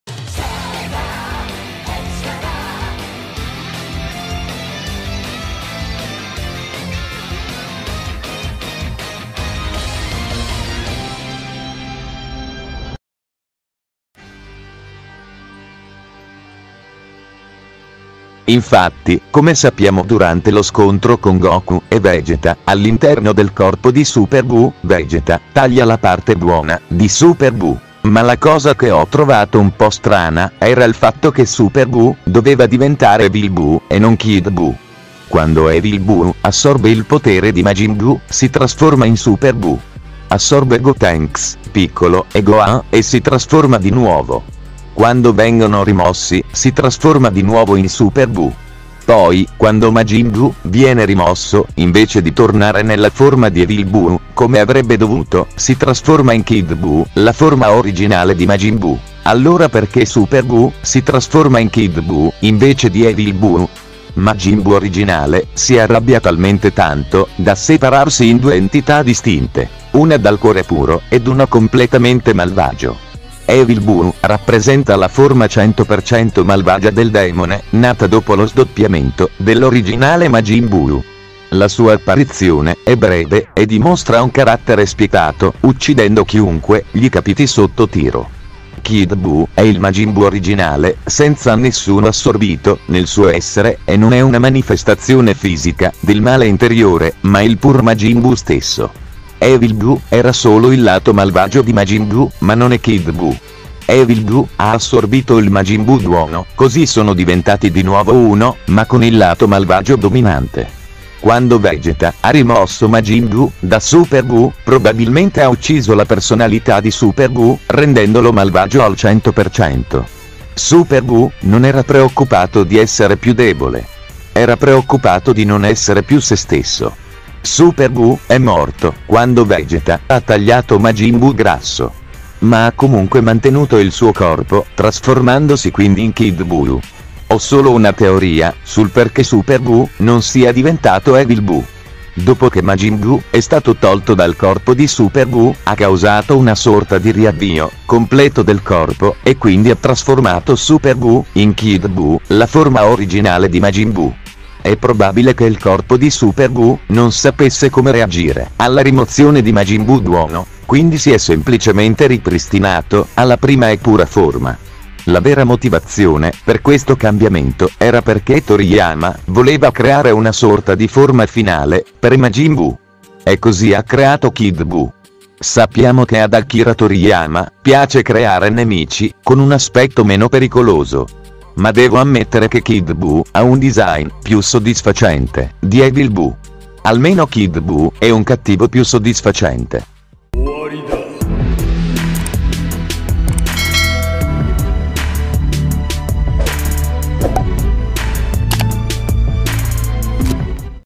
Perché Super Buu non è diventato Evil Buu?
Perché Super Bu, invece di diventare Evil Bu, diventa Kid Bu, scoprilo in questo video.